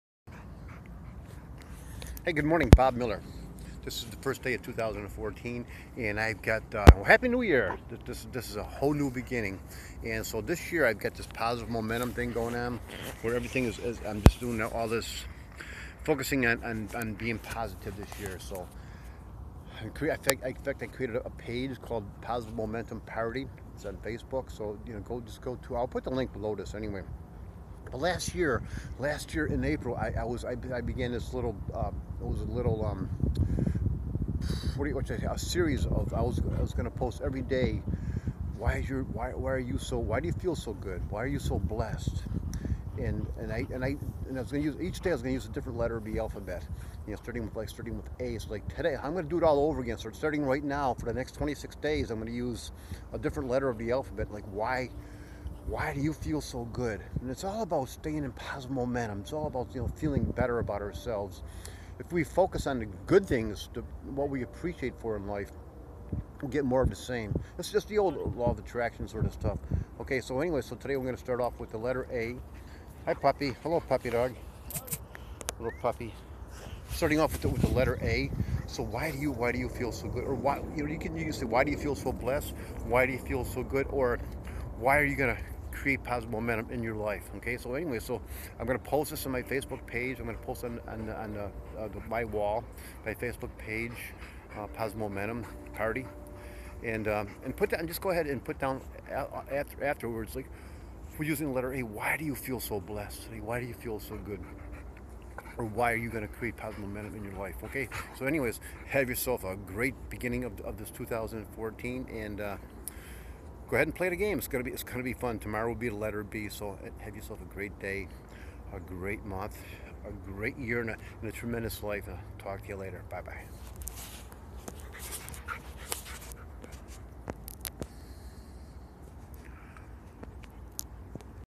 New Year 2014 Fiesta Island San Diego letter A
Happy New Year. Each day I will create and post a video talking/asking about "Why is your life so blessed." "Why are you so happy" or "How are you creating positive momentum in your life?" Today, Jan. 1 2014 I used the letter A. So why are you so blessed starting with the letter A? Each day I will use another letter of the alphabet.